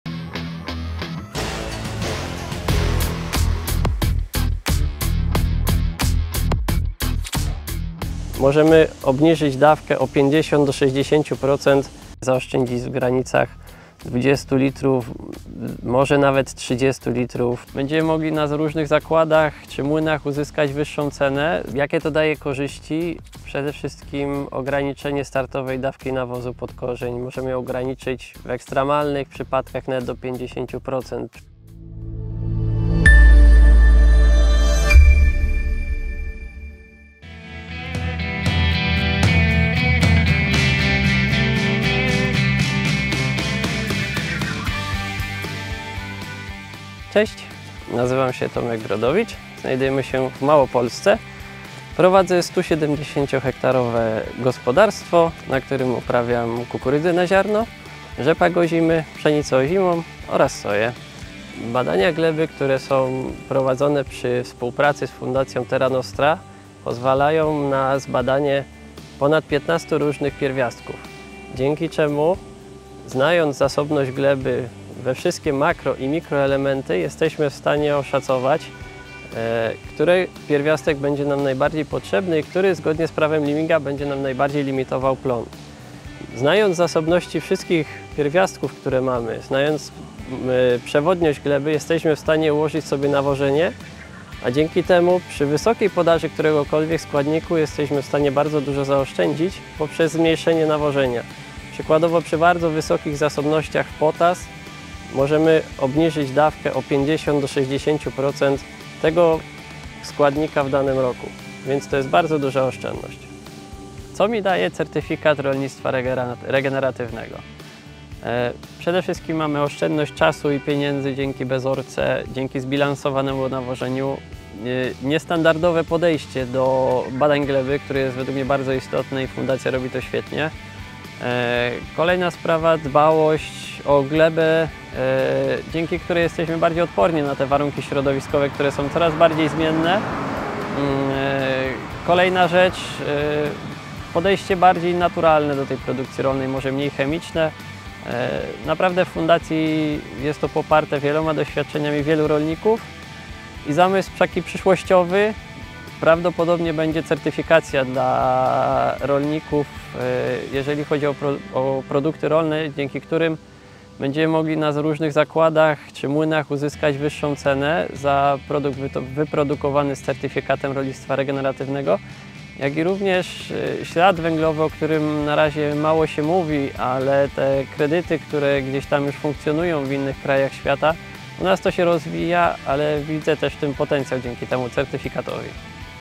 Rolnictwo regeneratywne w woj. małopolskim
🧑‍🌾🪱🚜Tym razem w ramach naszego cyklu filmów pokazujących gospodarstwa, których właściciele stosują zasady rolnictwa regeneratywnego odwiedziliśmy Pana Tomasza Brodowicza, który prowadzi gospodarstwo w województwie małopolskim, a w tym roku uzyskał certyfikat naszej fundacji. 💪💚Agronom uprawia pszenicę ozimą, rzepak ozimy, kukurydzę na ziarno, jęczmień jary oraz soję.
To 1 z 3 filmów, które wspólnie nagraliśmy, czeka na Was spora dawka informacji dotyczących zalet, specyfiki i korzyści wynikających z rolnictwa regeneratywnego.